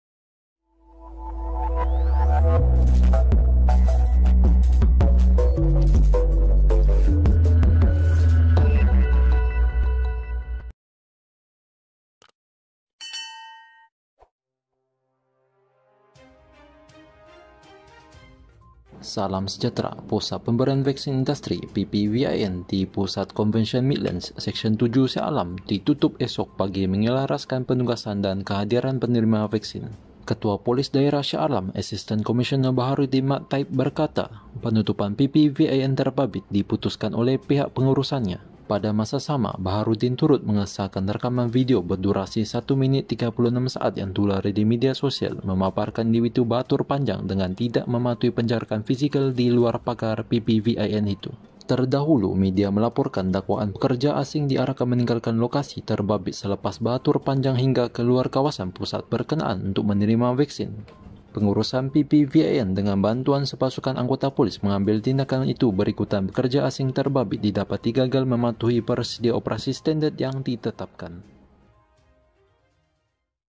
PPVIN TERPAKSA TUTUP!! TIDAK MAMPU TAMPUNG LAGI!!!
Pusat Pemberian Vaksin Industri (PPVIN) di Pusat Konvensyen Midlands, Seksyen 7, Shah Alam, ditutup esok bagi menyelaraskan penugasan dan kehadiran penerima vaksin